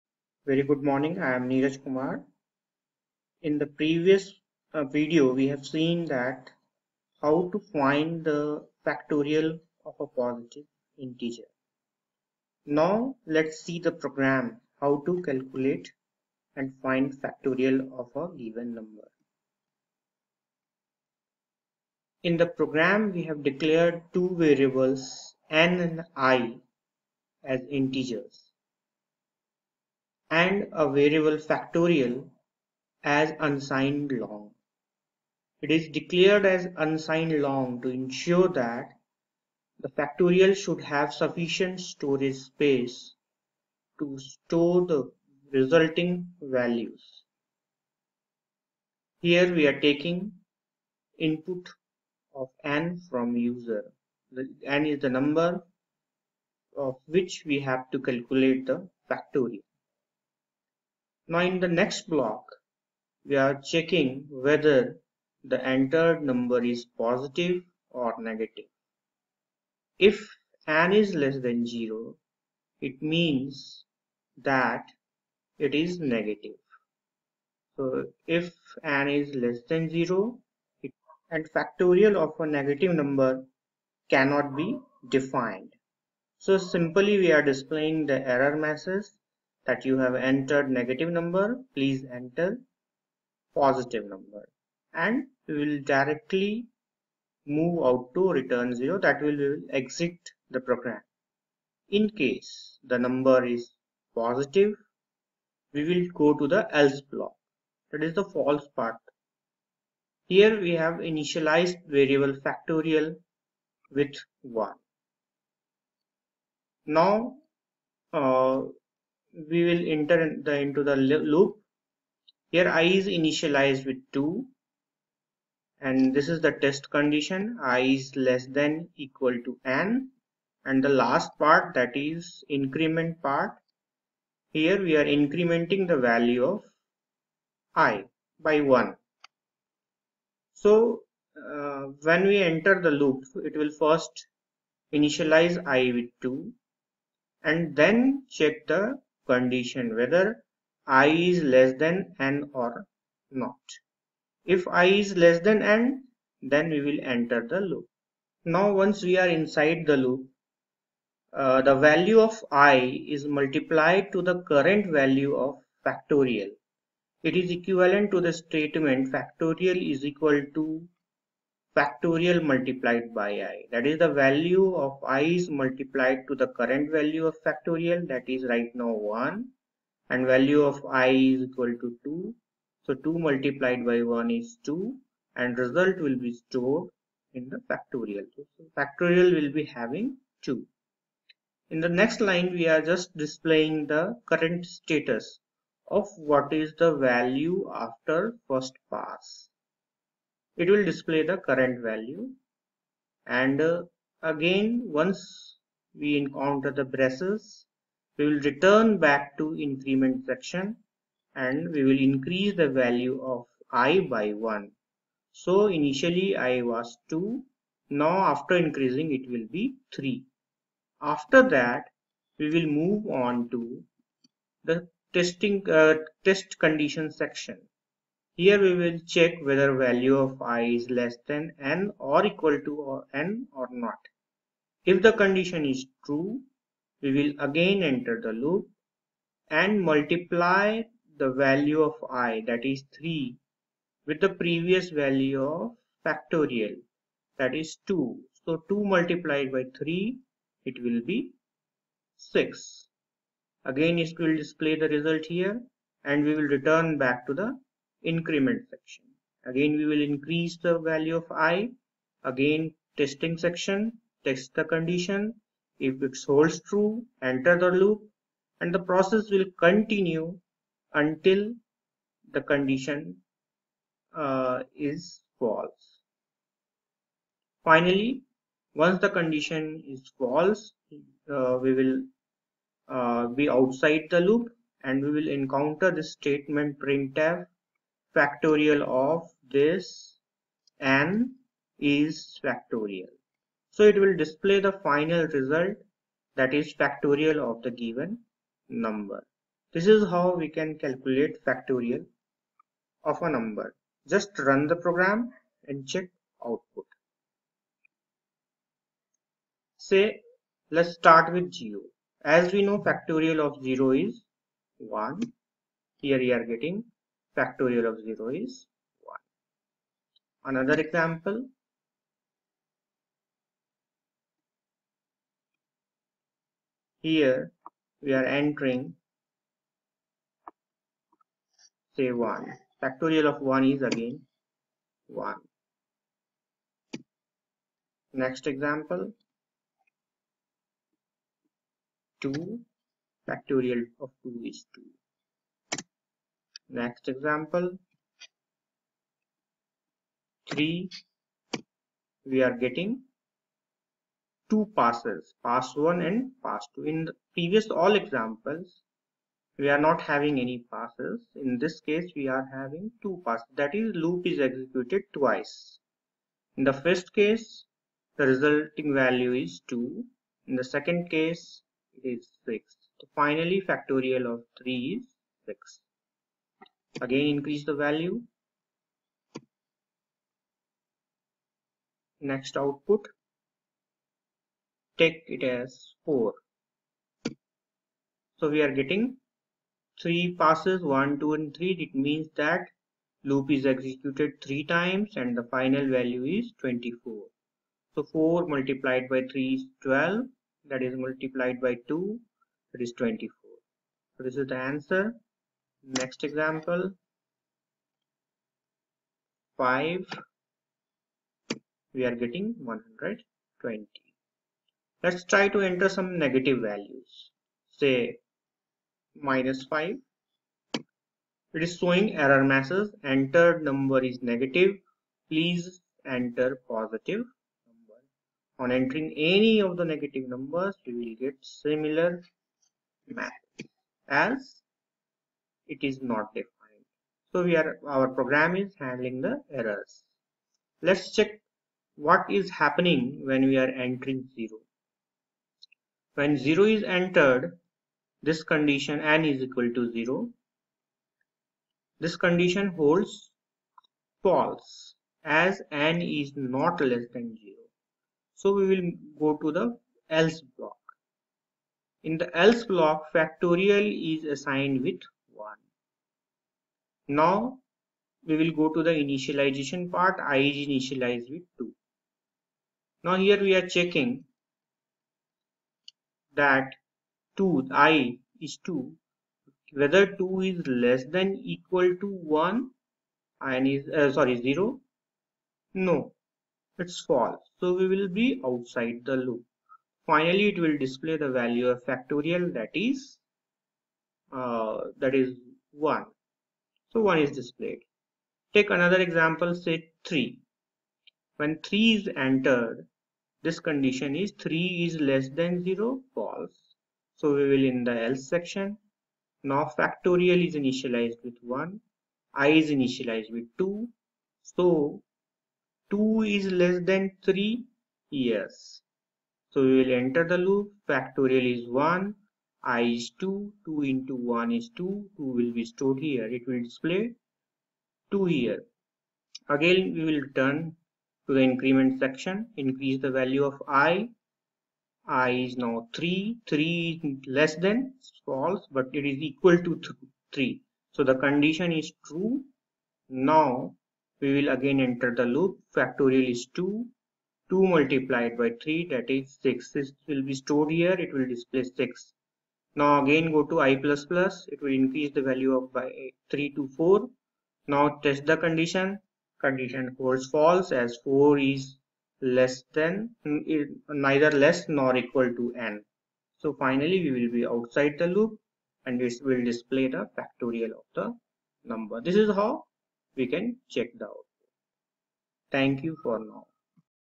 Program to Find Factorial of Positive Integer
This video contains program to find factorial of positive Integer.